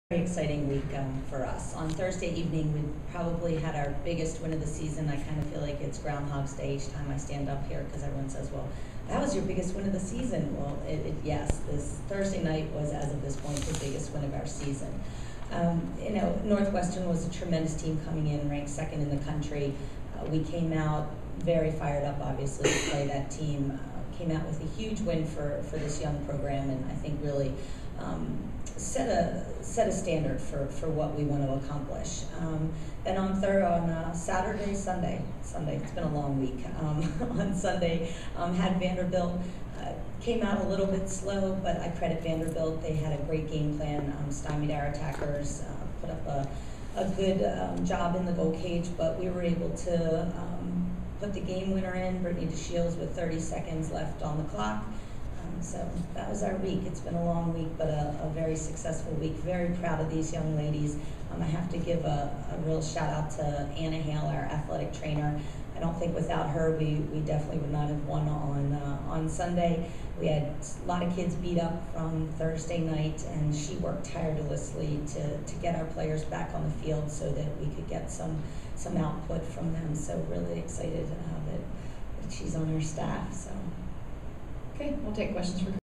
Amanda O'Leary talks big weekend for Gator lacrosse
Florida coach Amanda O'Leary discusses the Gators' first conference title and 14 game win streak.